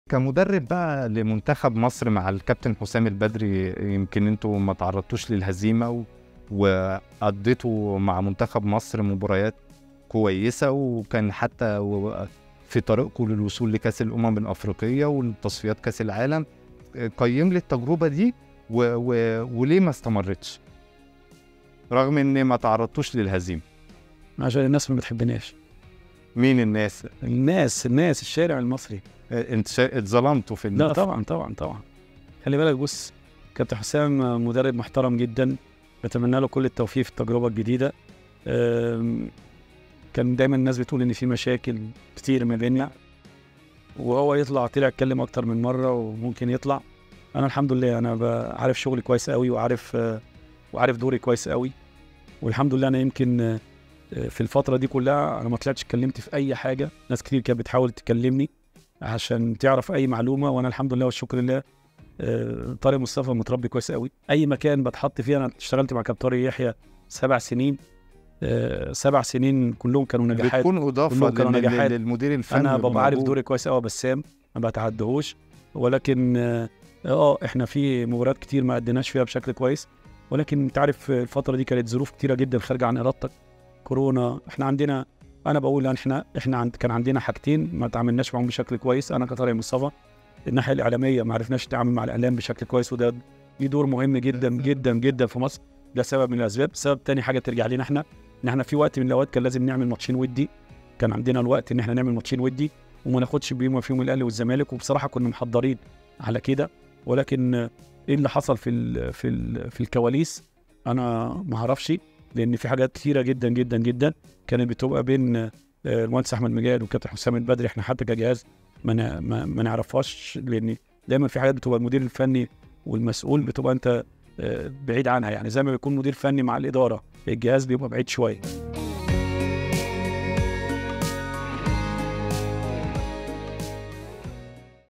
الشارع المصري لم يحبنا.. طارق مصطفى يوضح أسباب رحيله عن الجهاز الفني لمنتخب مصر بقيادة حسام البدري
💬 "الشارع المصري لم يحبنا"
طارق مصطفى يوضح أسباب رحيله عن الجهاز الفني لمنتخب مصر بقيادة حسام البدري 🗣️